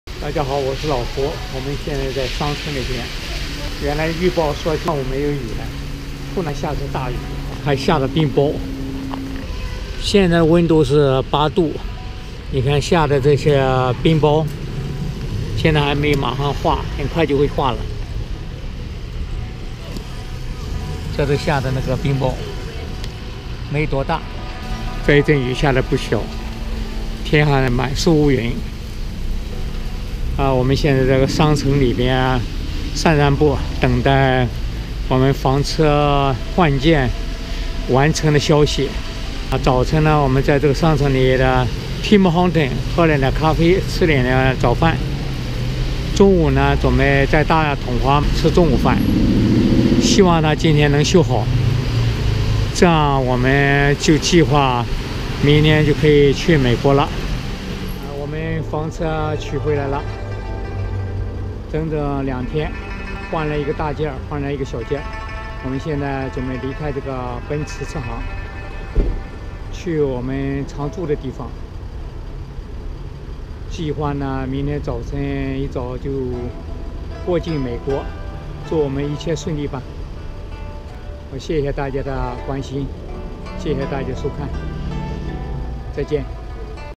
Sky suddenly change, Hail and Heavy Rain 天突然变脸没影响修好我的车, 追梦者胡 3012021
Sky suddenly change,  Hail and Heavy Rain
 天突然变脸没影响修好我的车,
追梦者胡